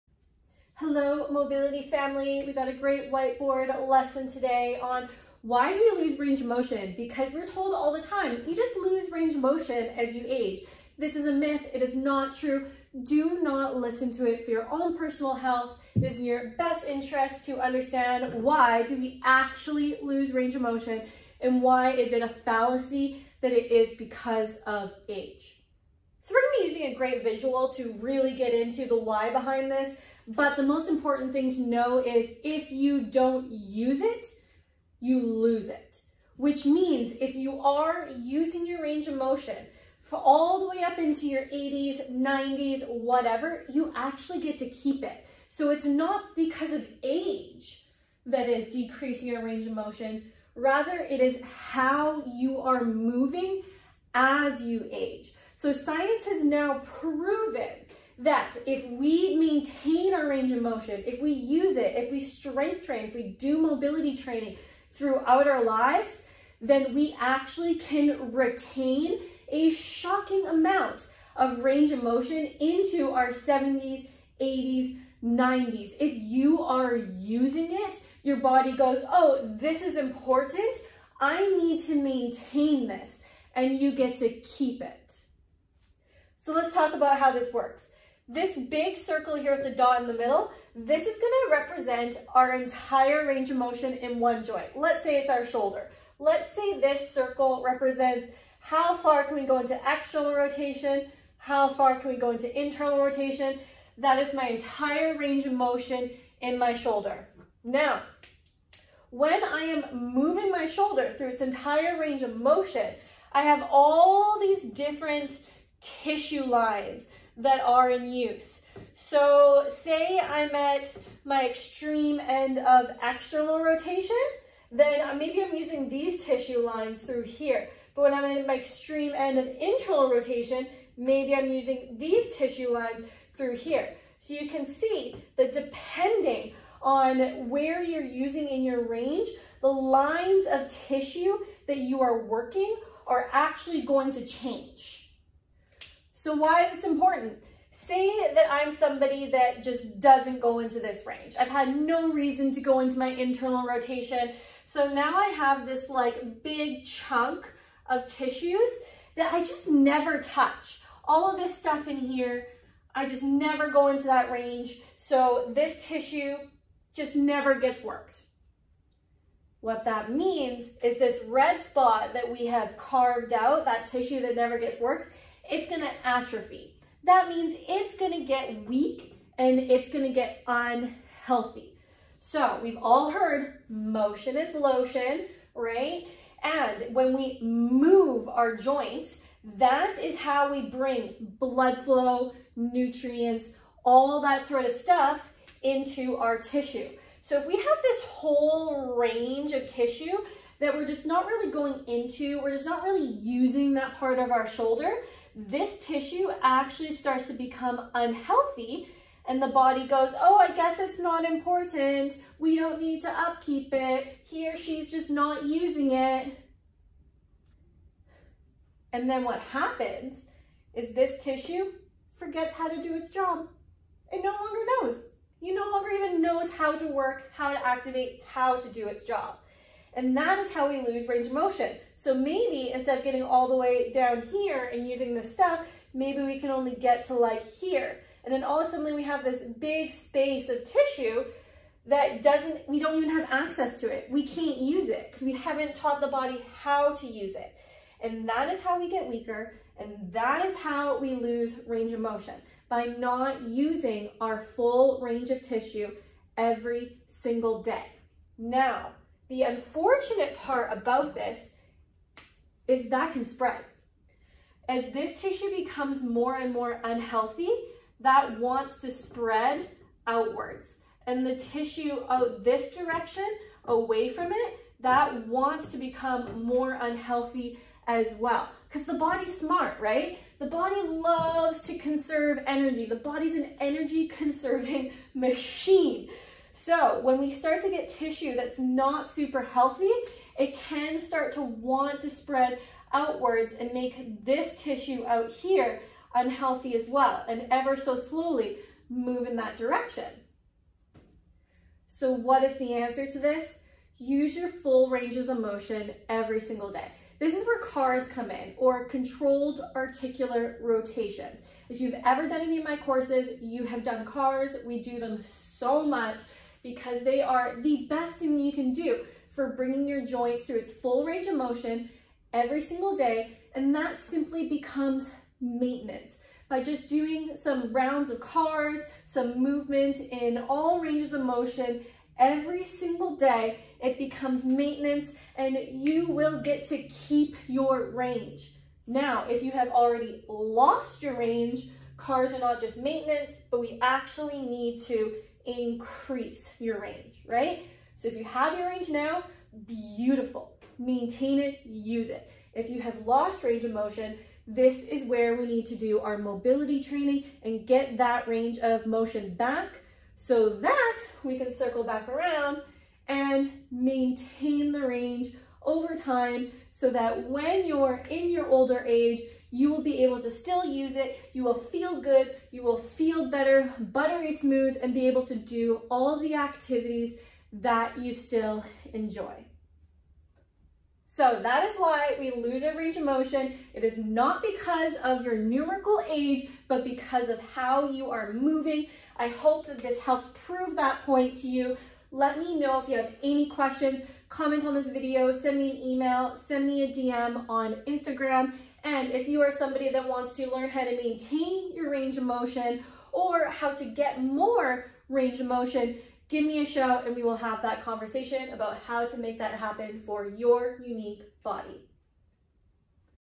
Why we lose range of motion - IT'S NOT B/C OF AGE
If you have been told you loser range because of age, this is a myth! Checkout why this is in the video above.